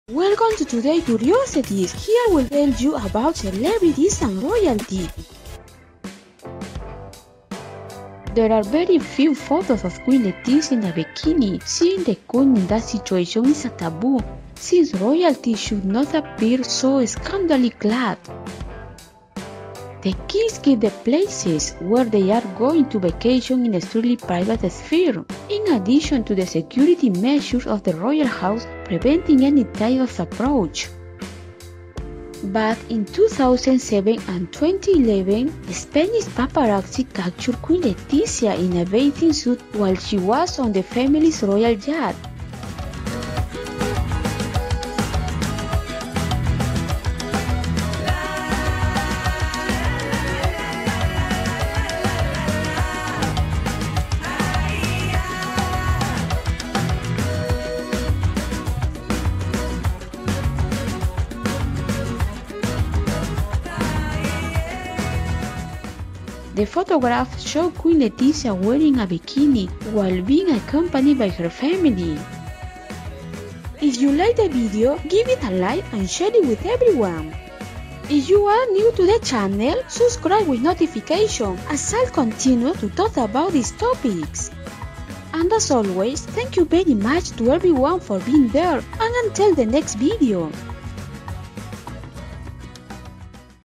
Check out the hot Queen Letizia in a Bikini on the royal yacht!
Queen Letizia in a bikini. Royalty  family are going to vacation in a strictly private sphere. But in 2007 and 2011 Spanish paparazzi captured Queen Letizia in a bathing suit while she was on the family's royal yacht.